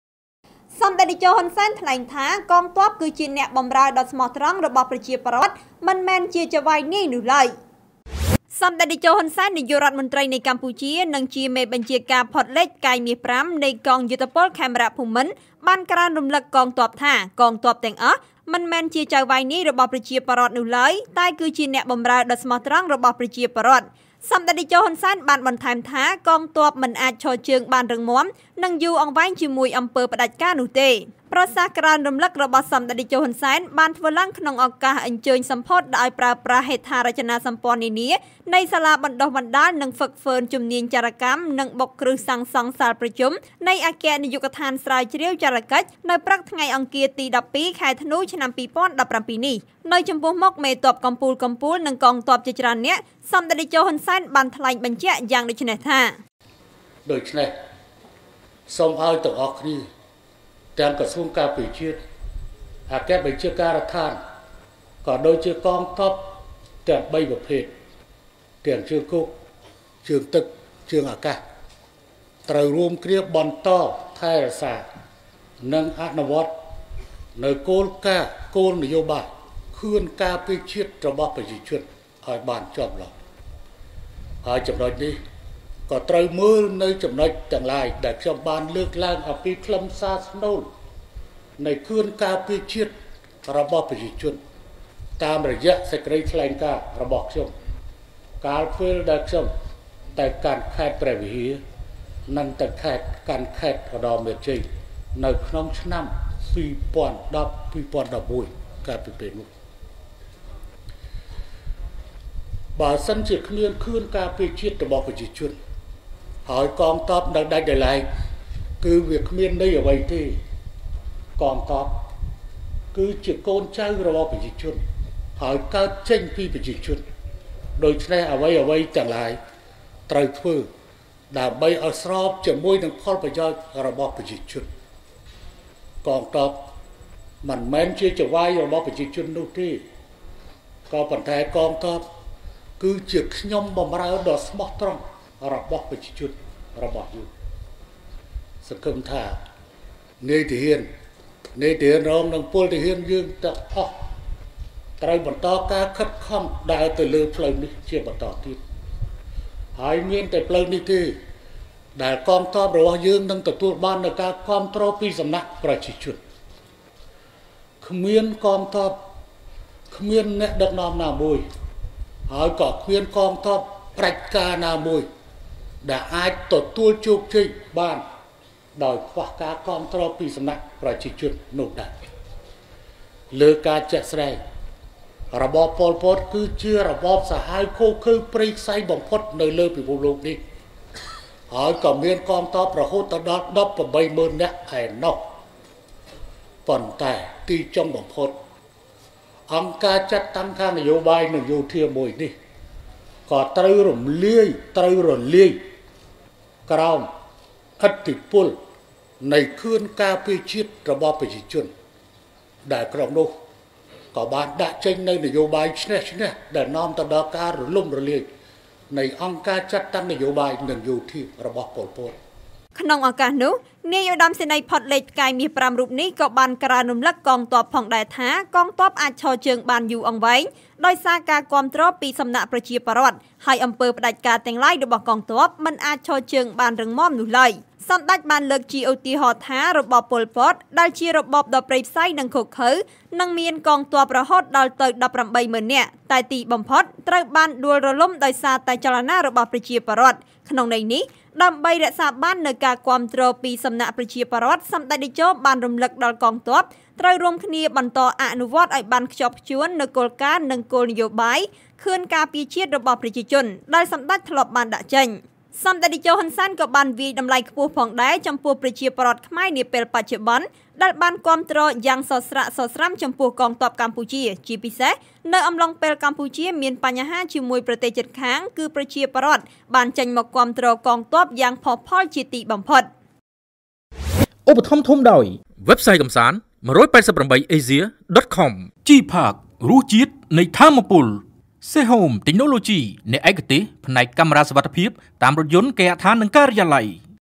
សម្តេចតេជោ ហ៊ុន សែន៖ កងទ័ពគឺជាអ្នកបម្រើដ៏ស្មោះត្រង់របស់ប្រជាពលរដ្ឋ មិនមែនជាចៅ...
សម្តេចតេជោ ហ៊ុន សែន៖ កងទ័ពគឺជាអ្នកបម្រើដ៏ស្មោះត្រង់របស់ប្រជាពលរដ្ឋ មិនមែនជាចៅហ្វាយនាយនោះឡើយ